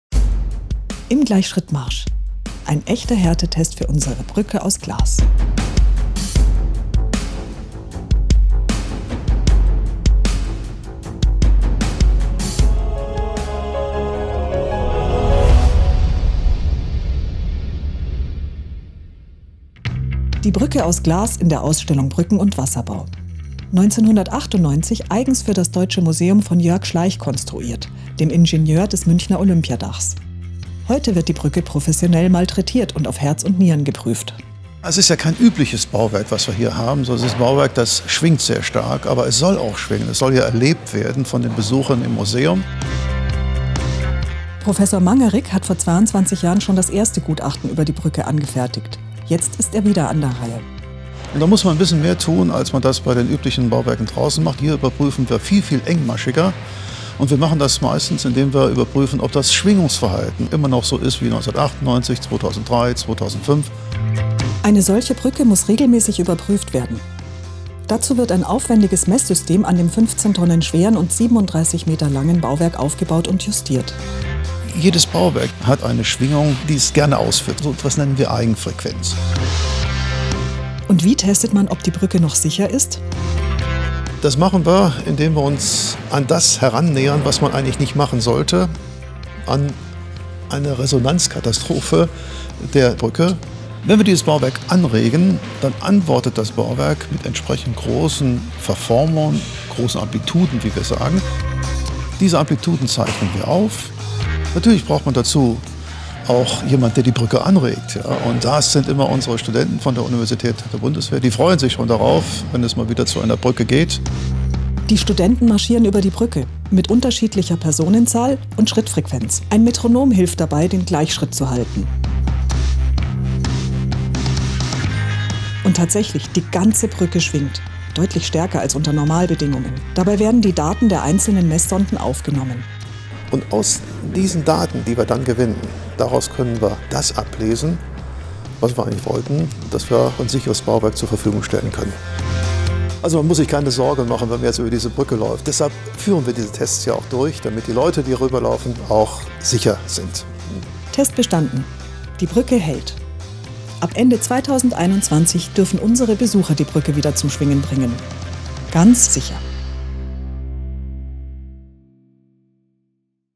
Der Härtetest
Im Gleichschritt Marsch: In der künftigen Brückenbau-Ausstellung wurde die vom Olympiadach-Ingenieur Jörg Schlaich eigens für das Deutsche Museum konstruierte Brücke aus Glas einem echten Härtetest unterzogen. Im Video erfahrt ihr, was die Brücke dabei aushalten musste und warum dieser Test auf Herz und Nieren regelmäßig nötig ist.